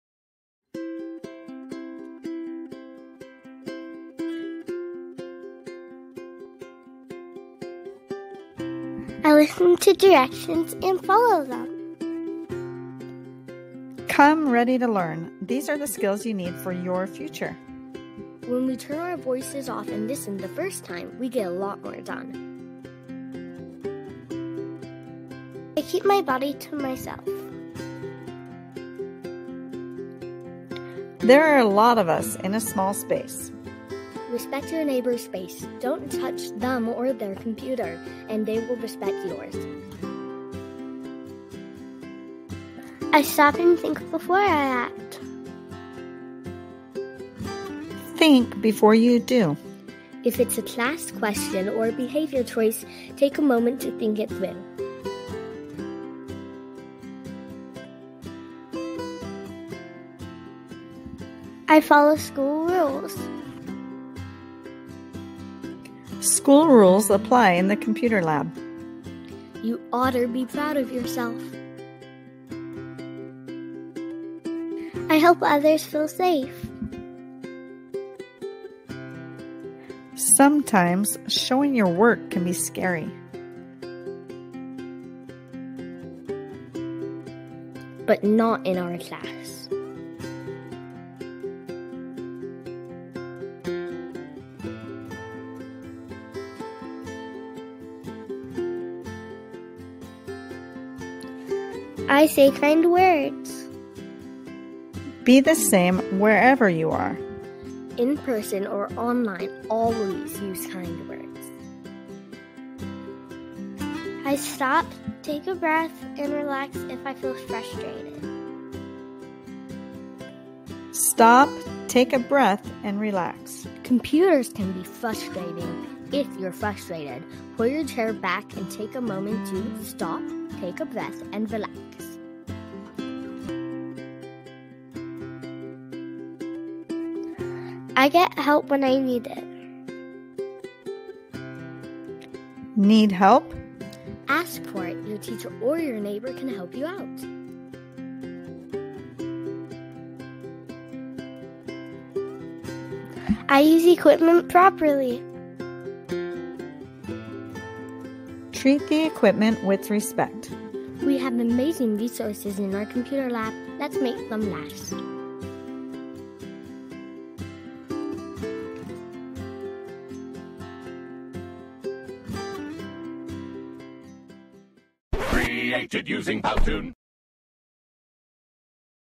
I Can Be Safe: Safety in the Computer Lab
In our computer lab, we focus on four behaviors: safety, respect, responsibility, and participation. This video talks about safety and the best ways we can be safe in our classroom!